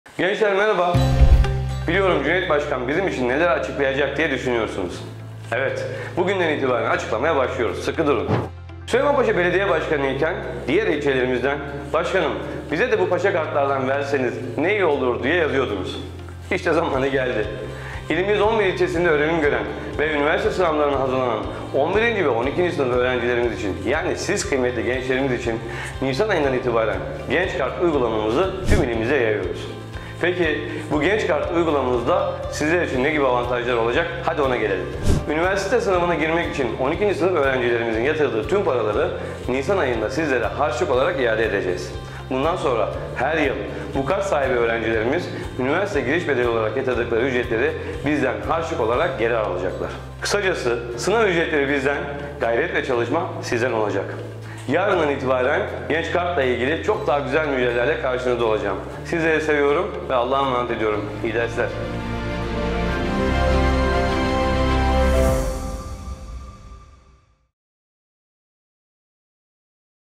Gençlerimiz İçin Genç Kart, Ayrıcalıklarla Geliyor! (1)
𝗦𝗲𝘃𝗴𝗶𝗹𝗶 𝗚𝗲𝗻𝗰̧𝗹𝗲𝗿!
Geçtiğimiz dönem Süleymanpaşa ilçemizde bir ilke imza atarak, üniversiteye hazırlanan öğrencilerimiz için “Paşa Kart” hizmetini hayata geçirmiştik.
Binlerce öğrencimize ücretsiz şehir içi ulaşımı, belediyemize ait kefelerimizde indirim ve haftalık internet hizmeti sunduğumuz projemizi, ilimizin tamamına yayacağız.

Tekirdağ’ımızda üniversiteye hazırlanan tüm öğrencilerimize vereceğimiz “Genç Kart”ın ayrıcalıkları videomuzda!

Şimdiden hayırlı olsun!